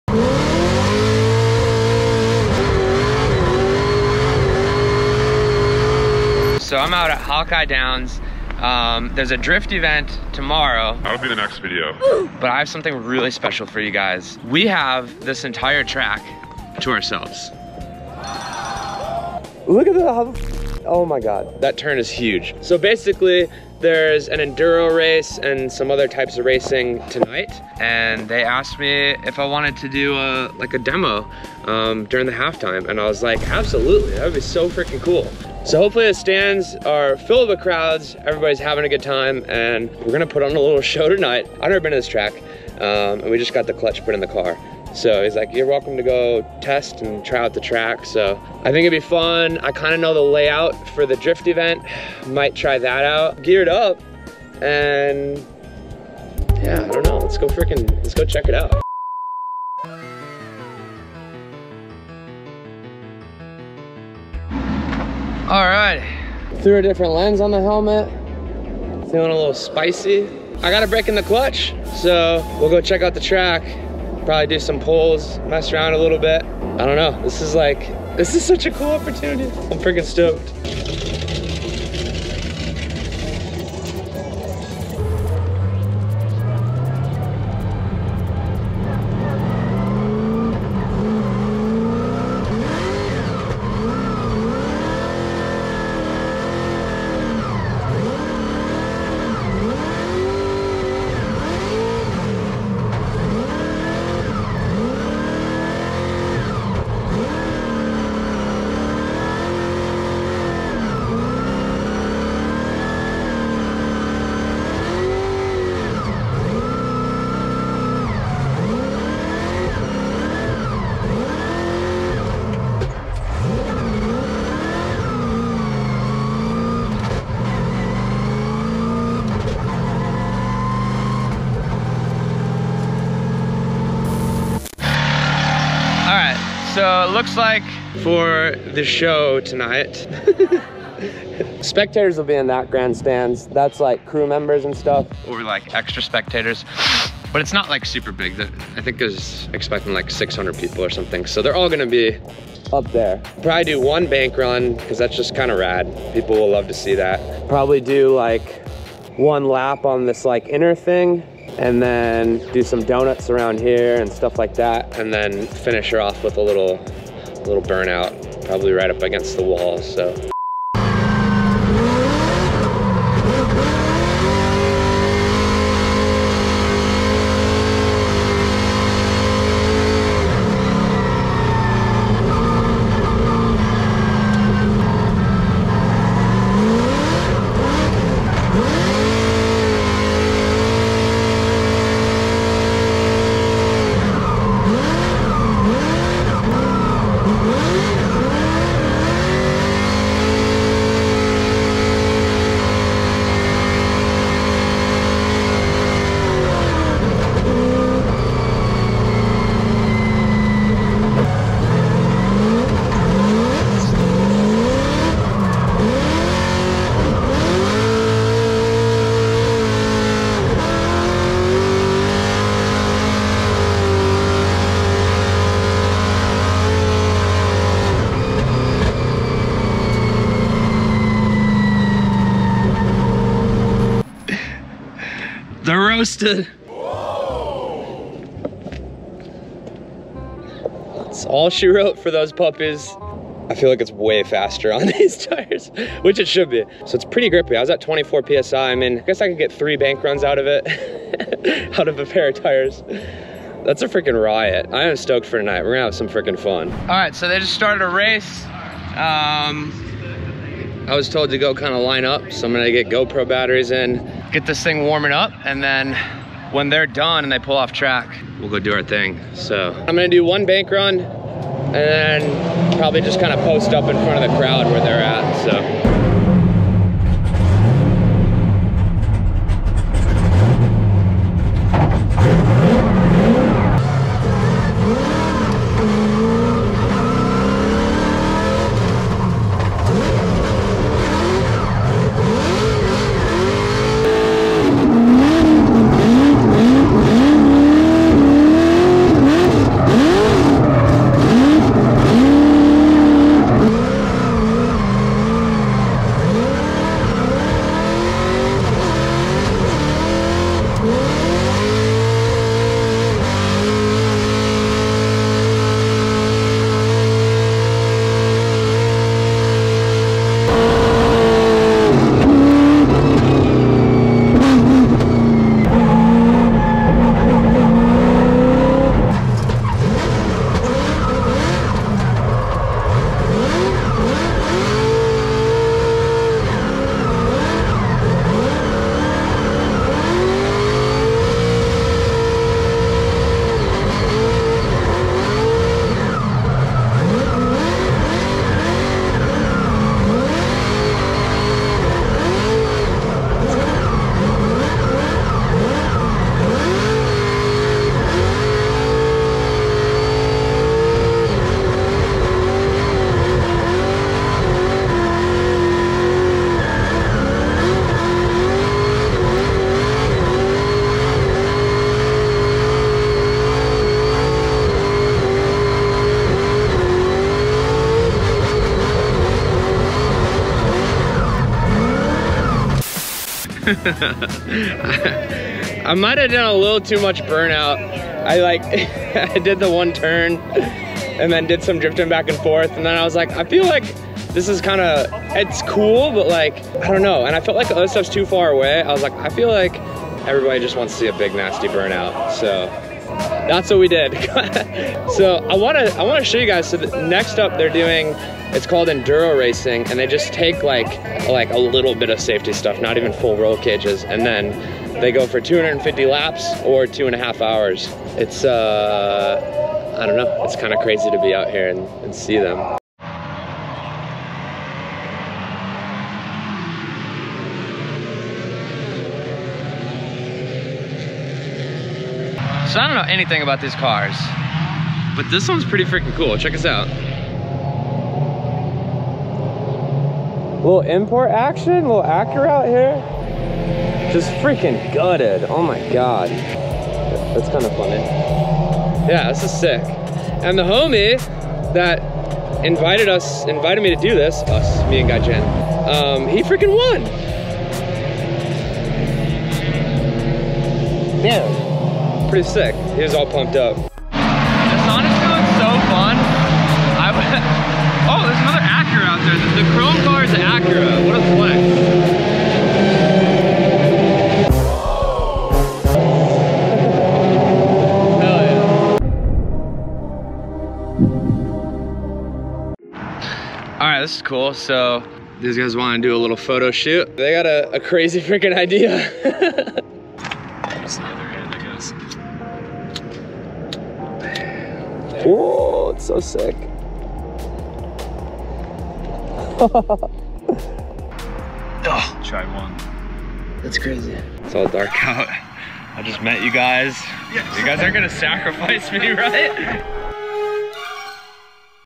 I Drift a halftime show at NASCAR Track!
I went to Hawkeye Downs, a NASCAR style circle track with fast banks, on my Drifting Across America Tour and I got to drift at the halftime show! This was such a rad experience with 136 mph wheel speed on 4th gear on the bank!